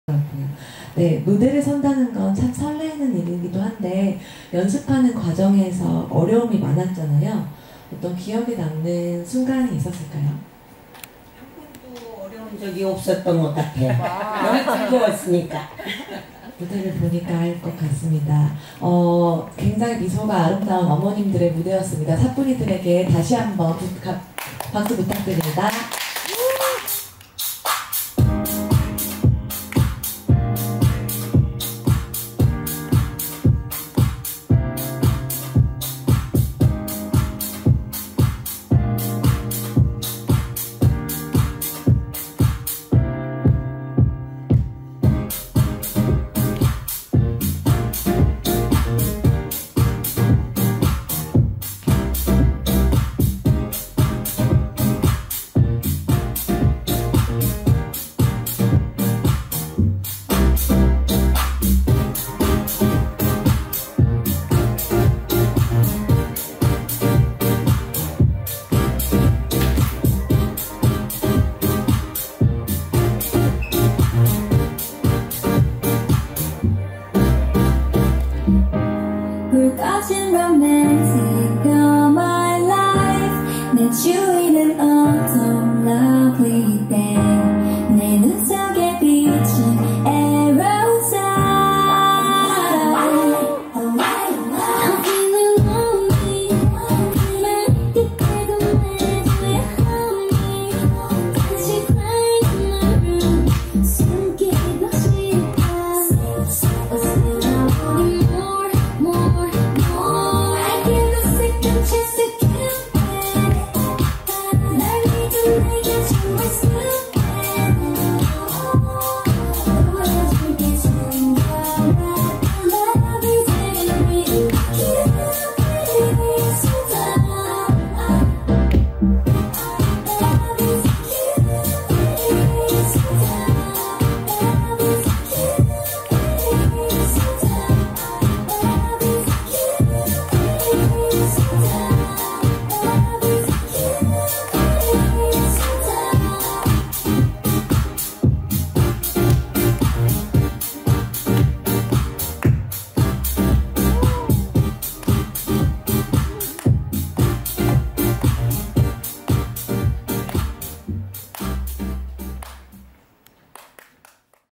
Korea House Fusion Connection Vol.1 | 사뿐이들
Korea House Fusion Connection Vol.1
사뿐이들
"사뿐사뿐 하우스댄스를 통해 만난 우리"
마음만큼은 청춘인 어머님들의 에너지 기대해주세요